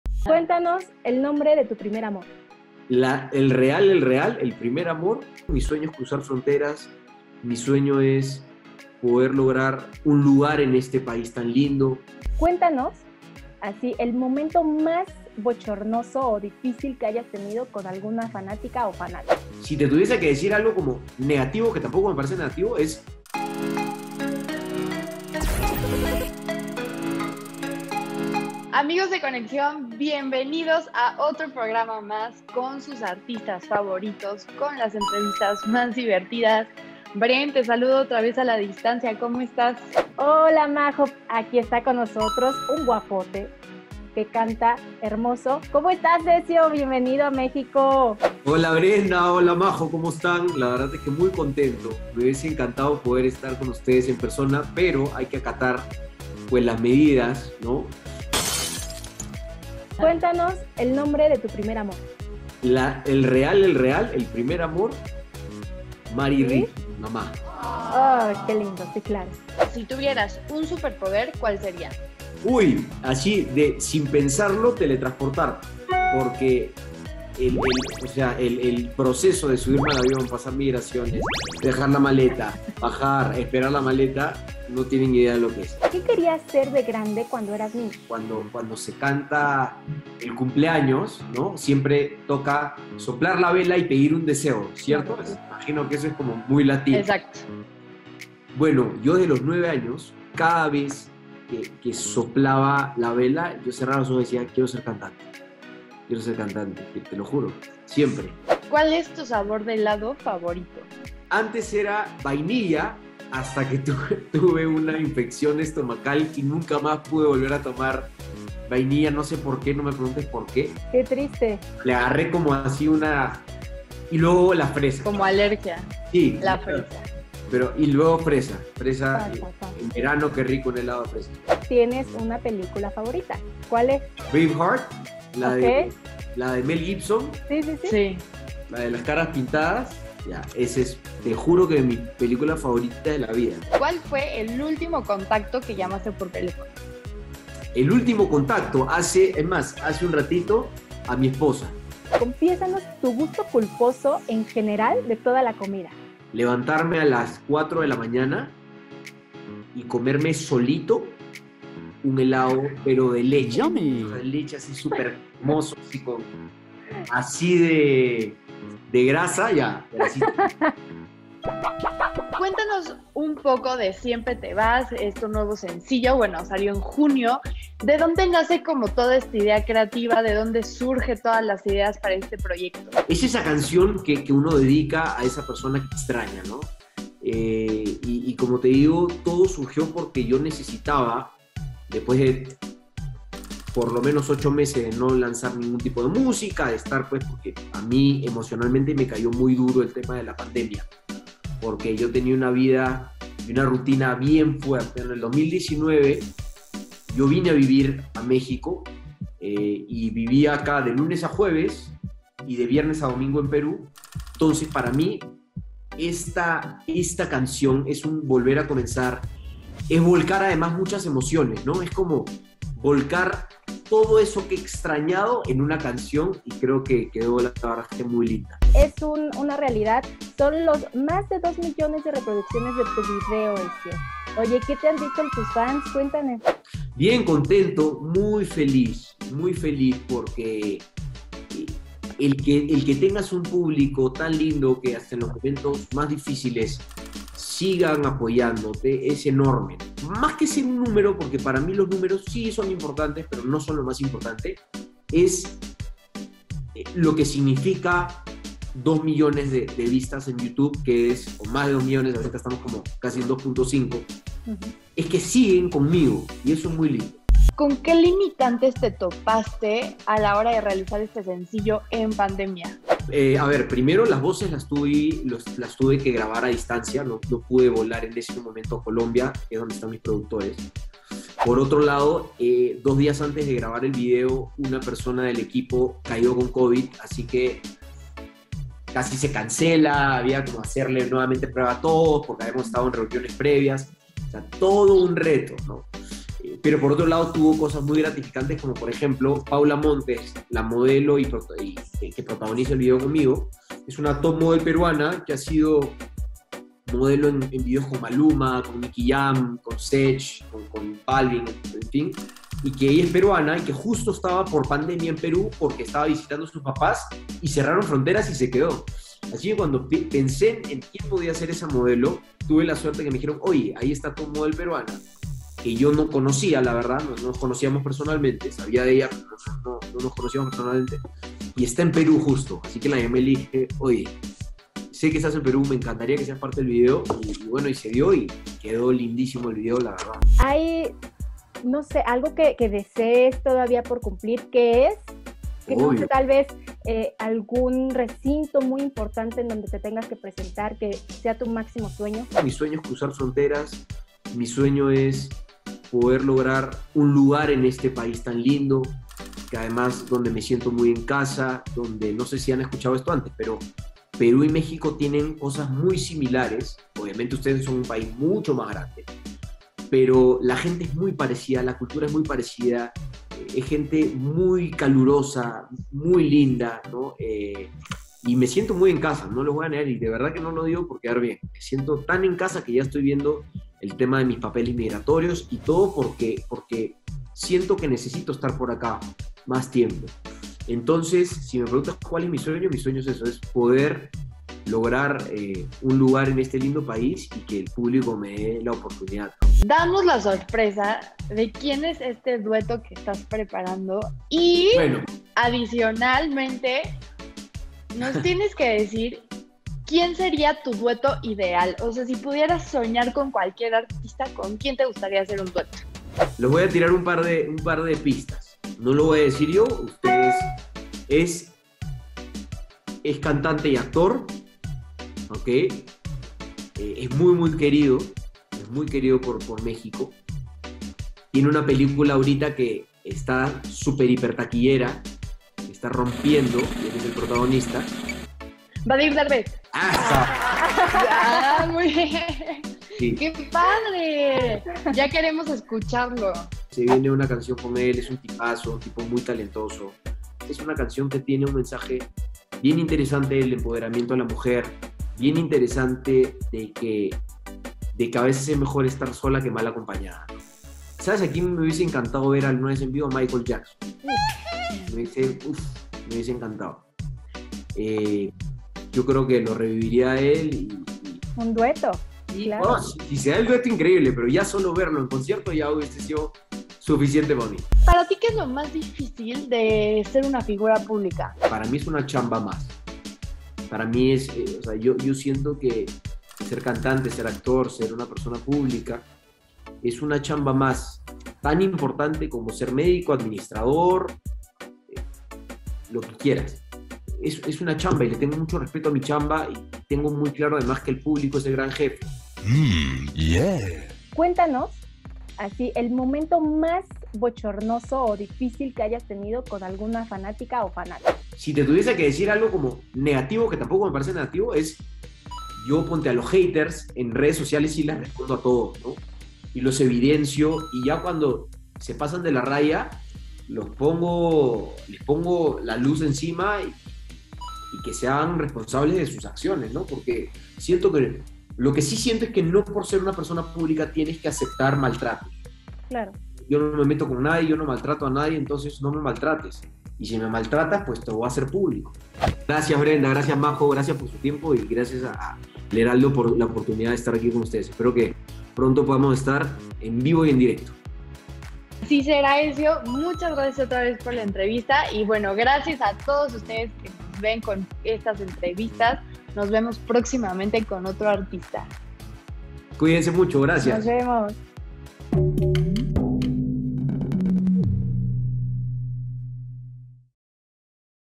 Ezio Oliva REVELÓ quién fue su PRIMER AMOR
ConexiónHeraldo | ¿Quién fue el primer amor de Ezio Oliva? 😱 Esto y muchos más nos confesó el cantante aquí👇🏼 🎤